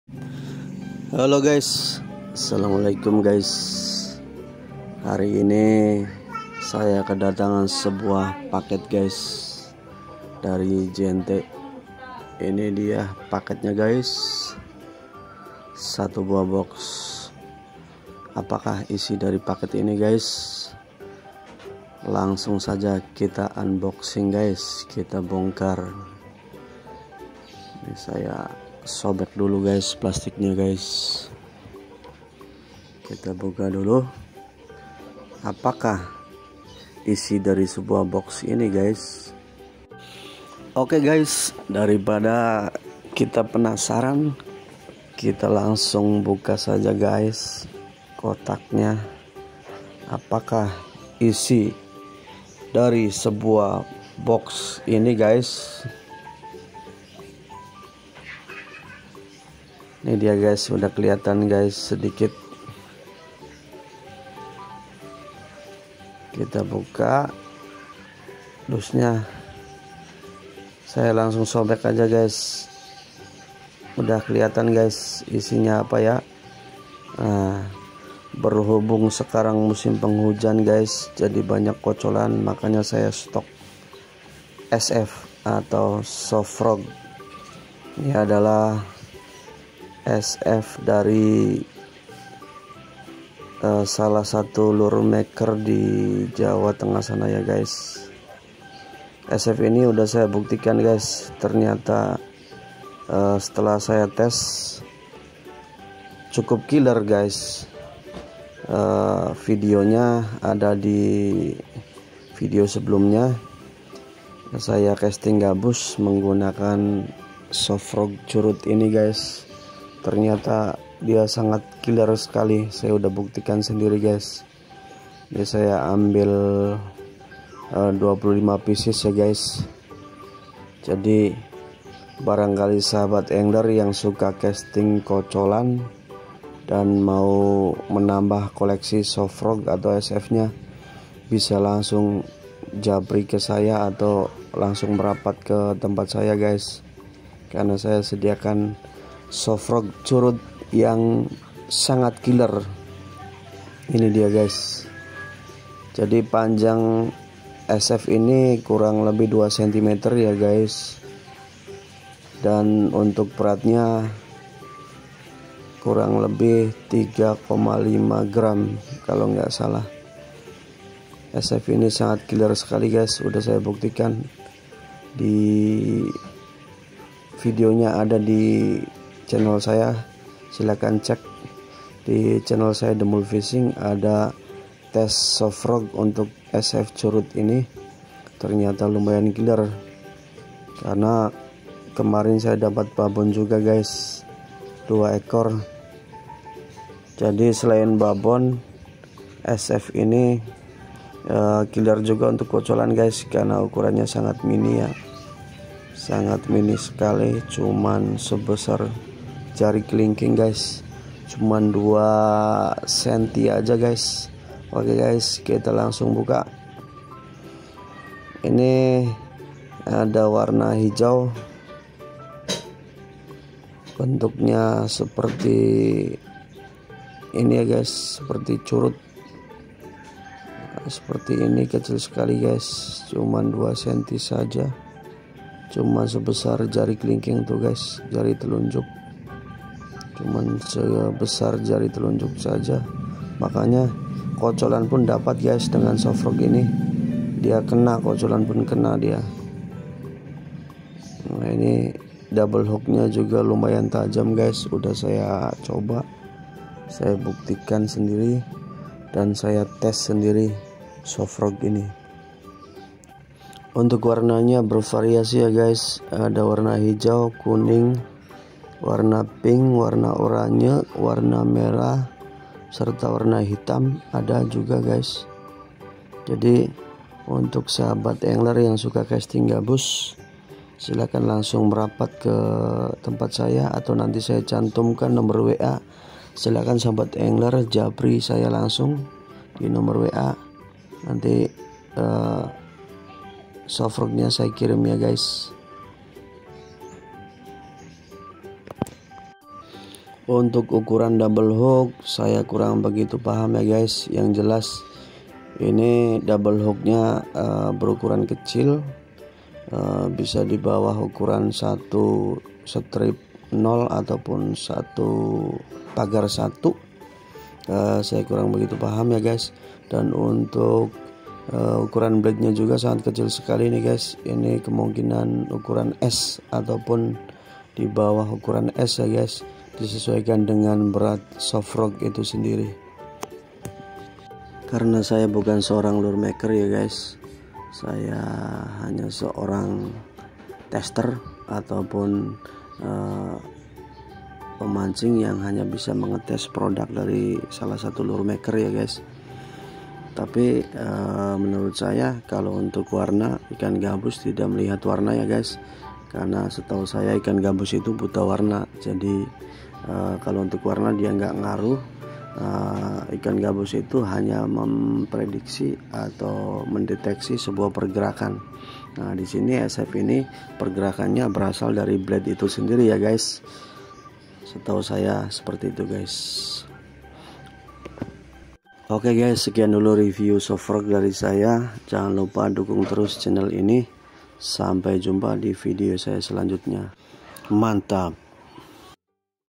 Unboxing Softfrog curut mini super killer 2cm 3,5gr❗
SF curut mini panjang 2cm , berat 3,5gr dgn hrg Rp.30.000/pcs  (recomended untuk set tackle ultralight)
Kontak pembelian di WA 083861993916